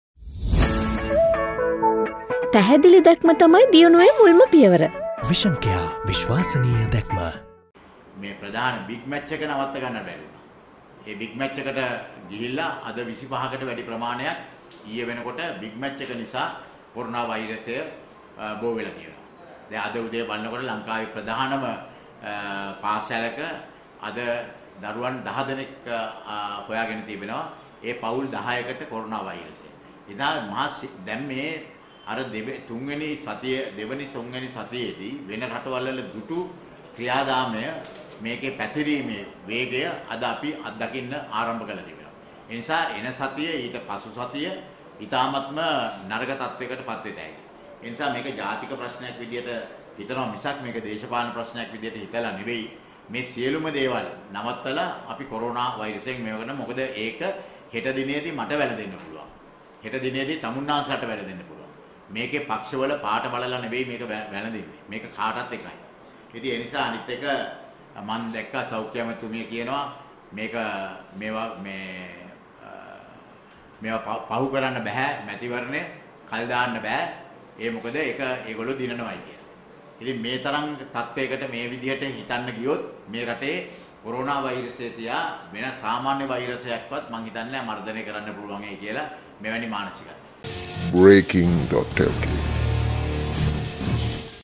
පාසල් සිසුන් 10කට කොරෝනා - රාජිත කියයි.| Breaking.lk
පාසල් සිසුන් 10කට කොරෝනා - රාජිත කියයි.